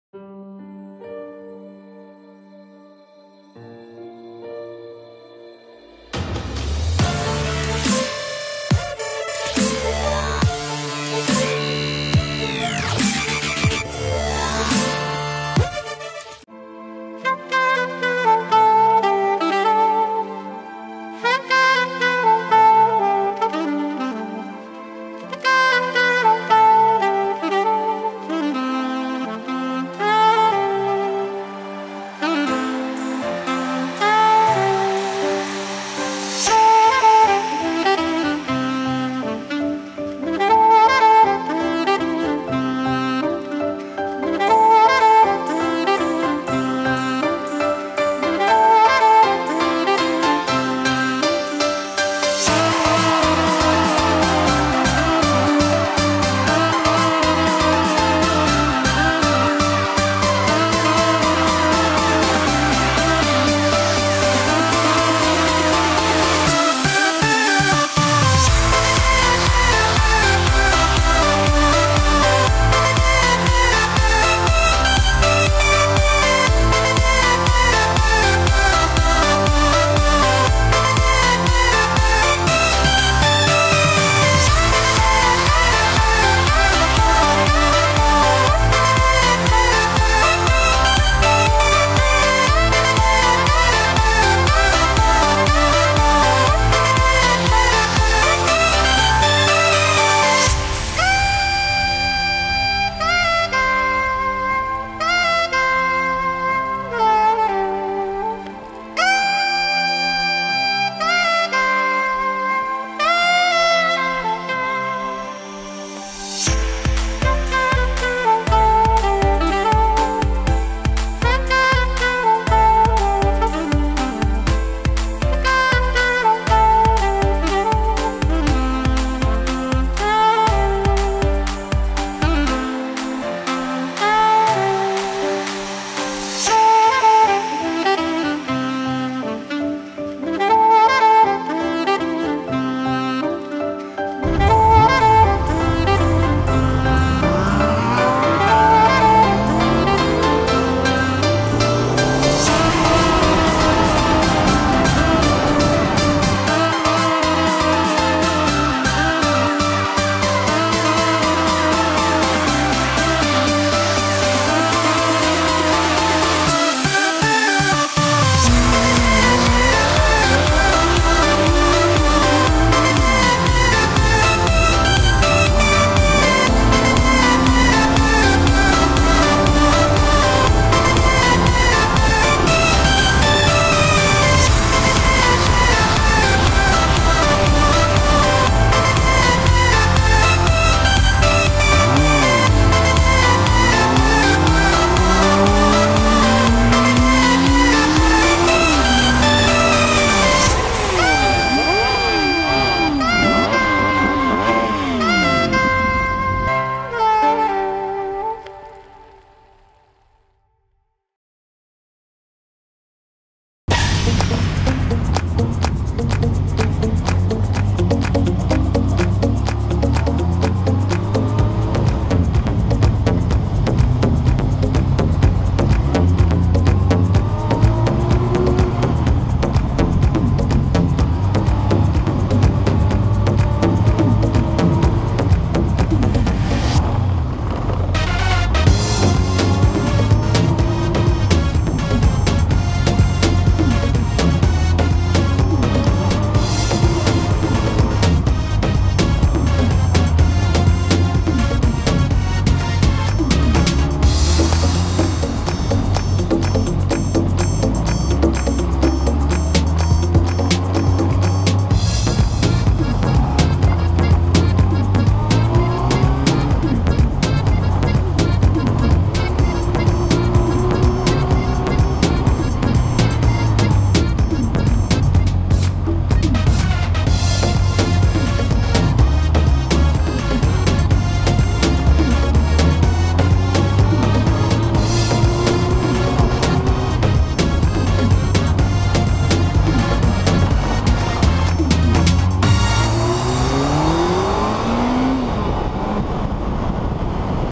Frankiee hat neue Freunde: Vinzler und Icke
Nachdem Ishimuro einen üblen Motorradunfall hatte, musste Frankiee sich neue Ausfahrkumpels suchen. Und warum kleckern, wenn man auch klotzen kann? Also schnappt er sich zwei berühmte Motovlogger und führt sie ein in die Welt der Musikvideos^^